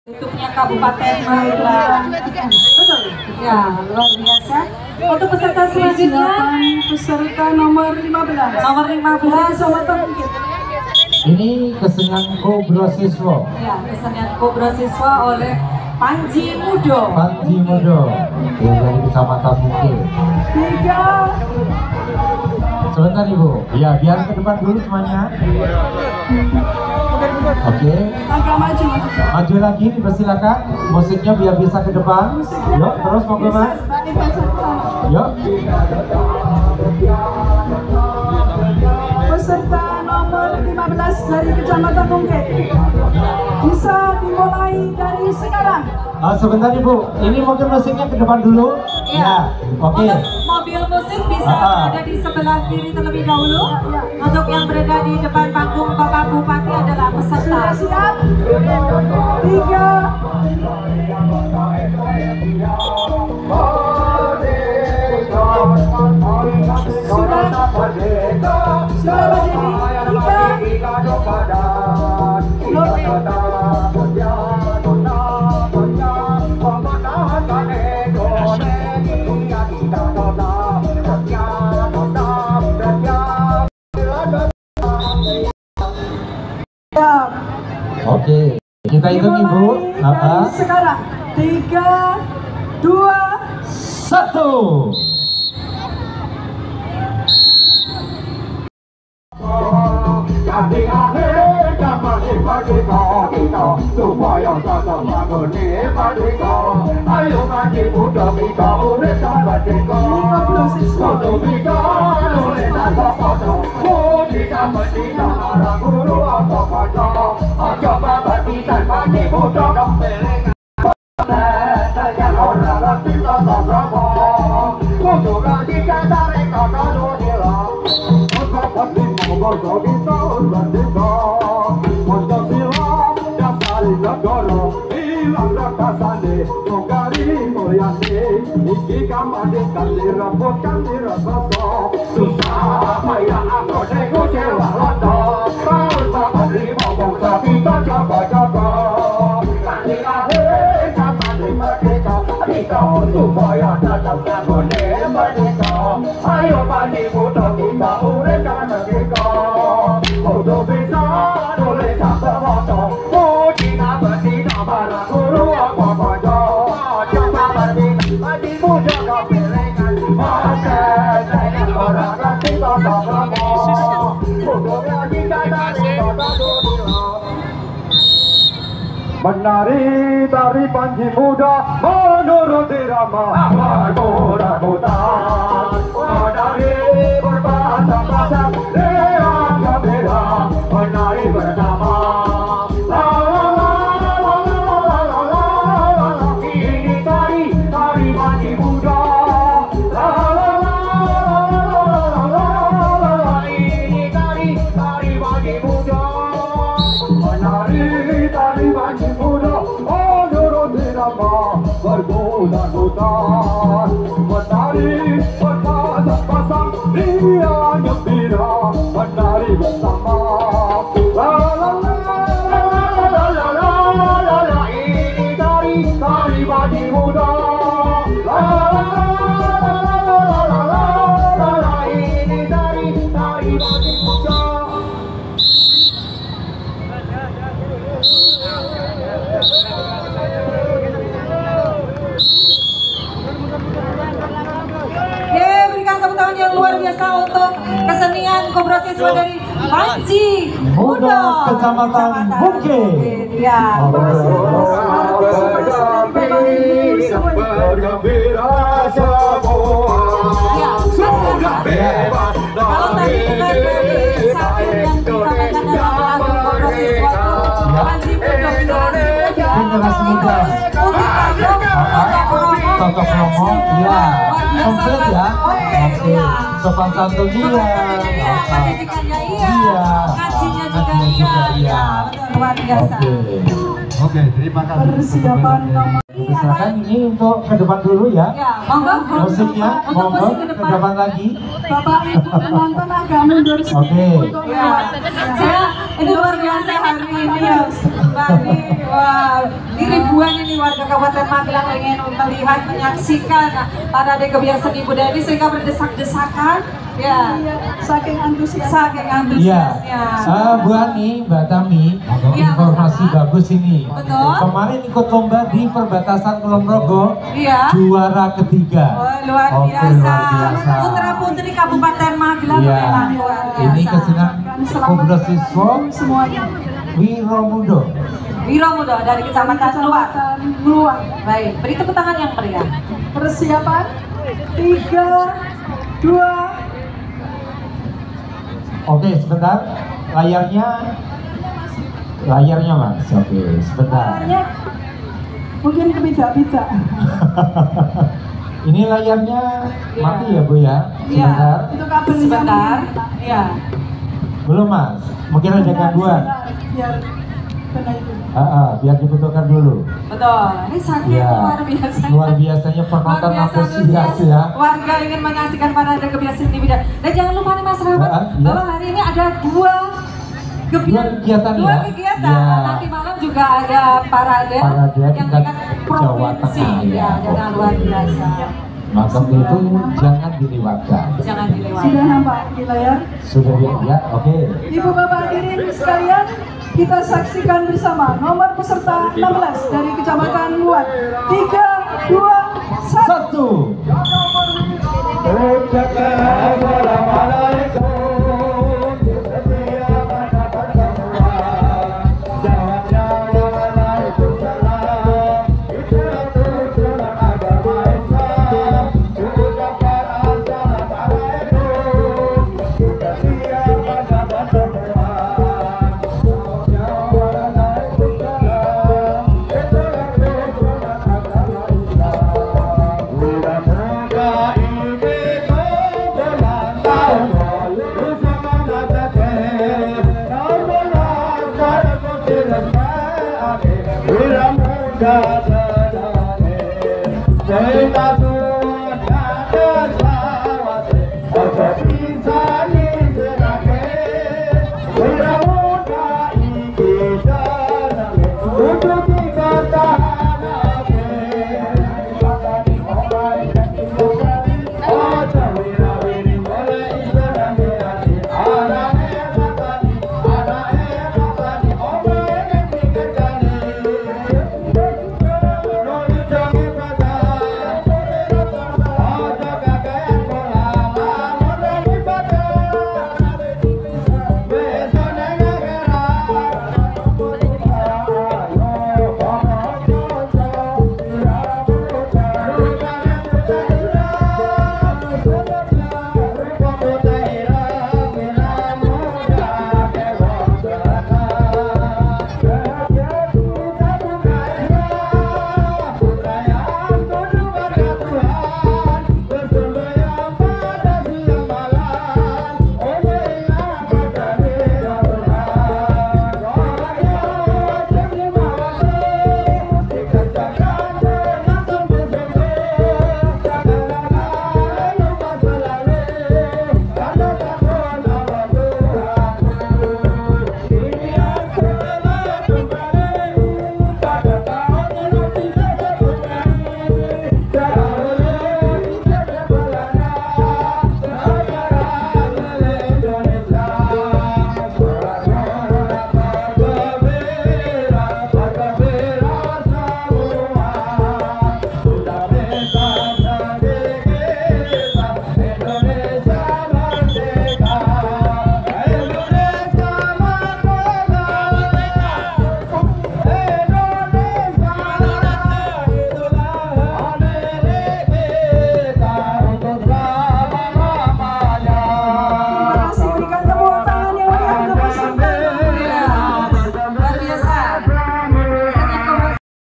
KOBRO SISWO KLASIK | PARADE GEBYAR SENI KOTA MUNGKID KABUPATEN MAGELANG 2023
perayaan HUT RI ke 78
Republik Indonesia